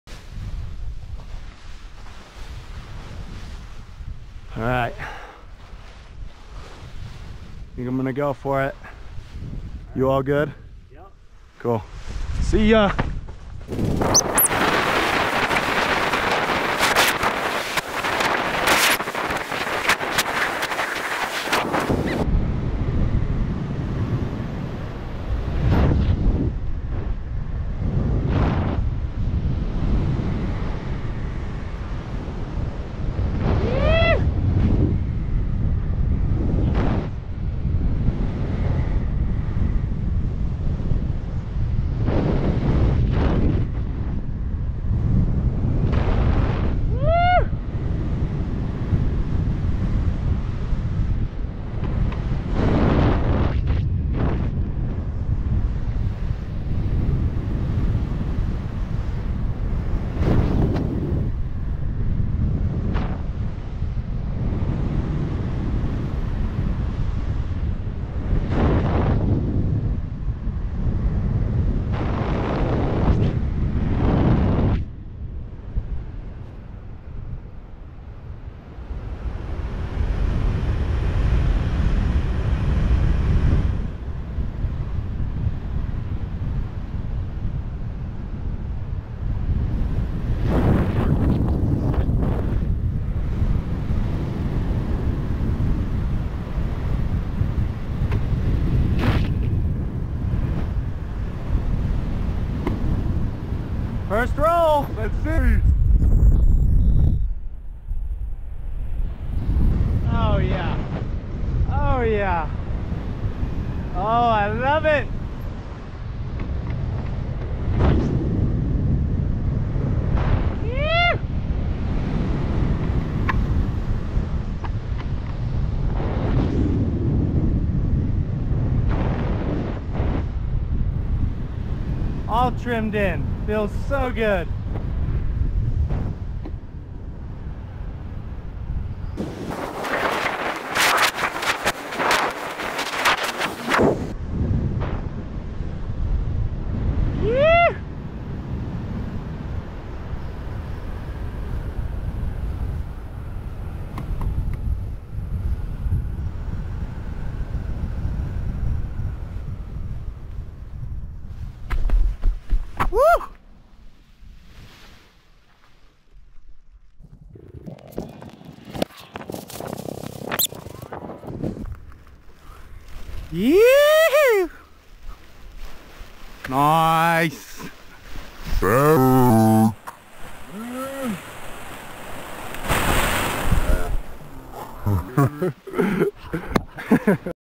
Speedflying Jobs Peak Swing Mirage RS+ 11m
First time flying my 11m Swing Mirage RS+. Sweet flight on Jobs Peak. Enjoy